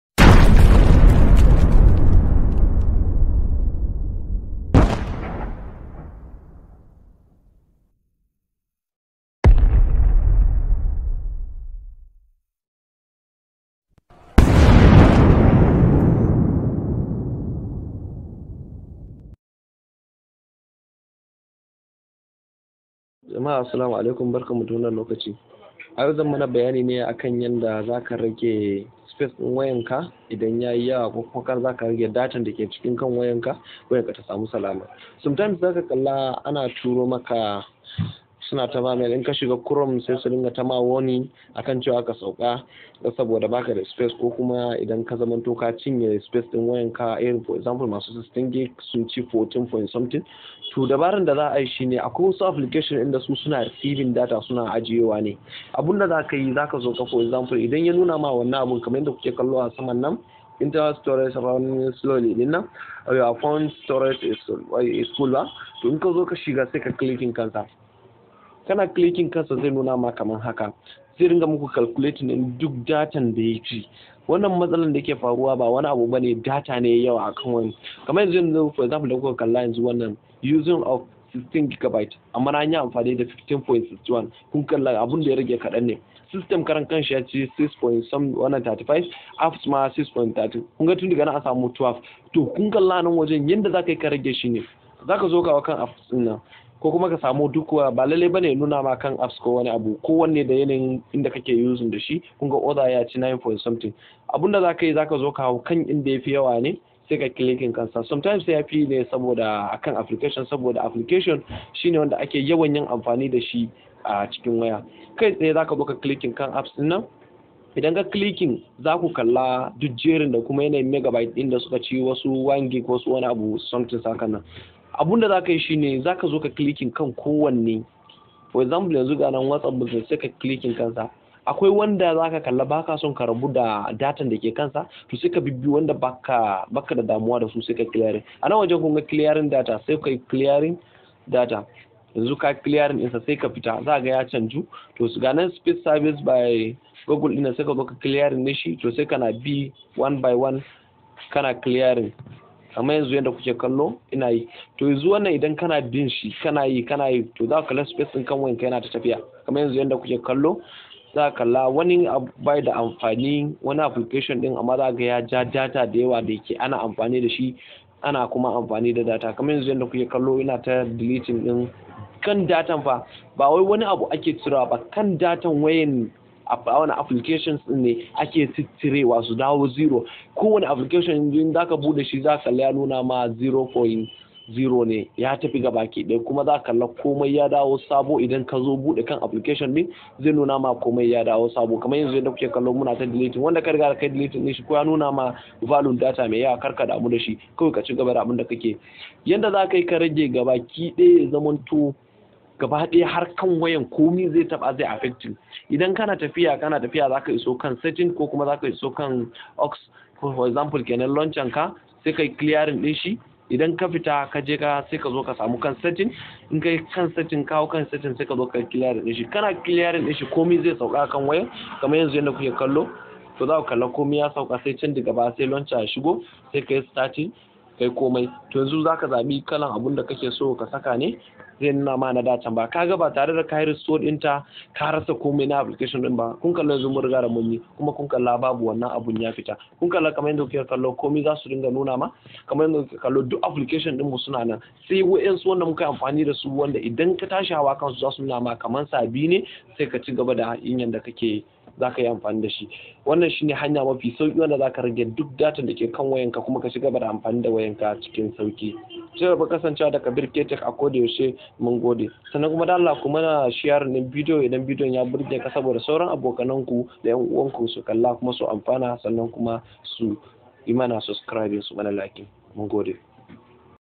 Yanda zaka rage space dinkan wayanka ba batare dakayi restore dintaba
aslm yen uwana ayau nayi mana bayanine akan yanda zaka rage space dinkan wayanka batare dakayi restore dintaba hanyane mafi sauki